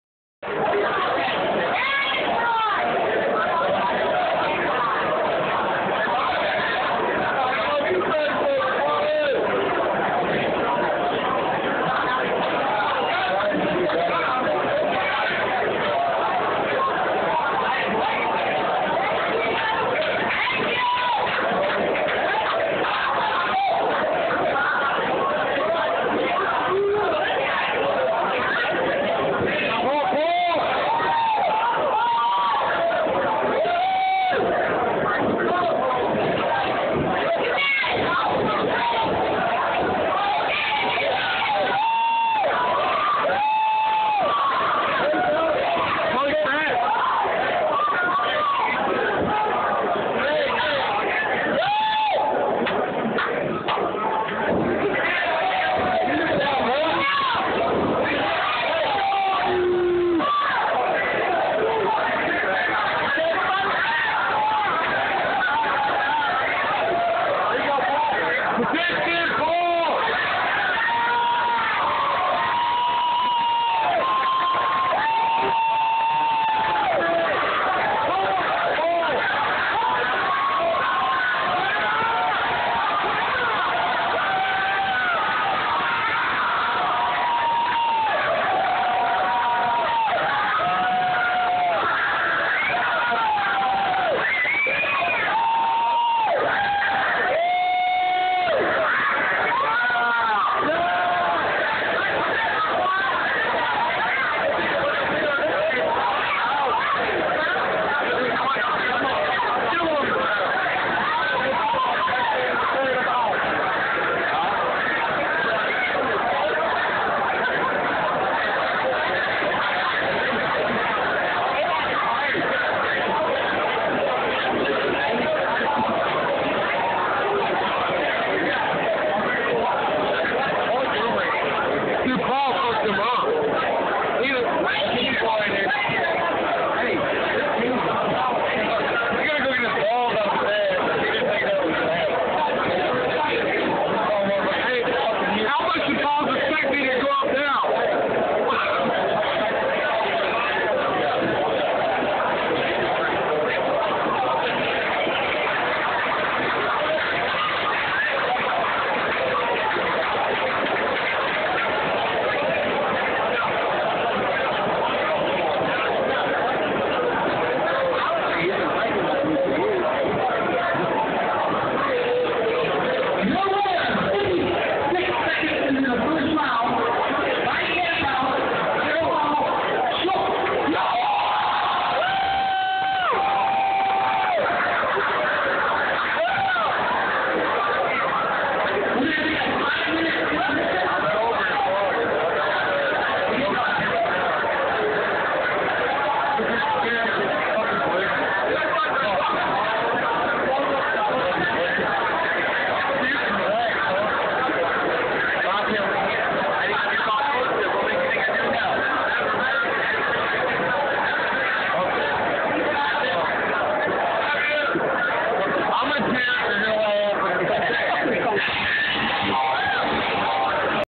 Paul Schultz vs Gerry Perry
My boy Paul Schultz dominates this chump in his first ever MMA fight.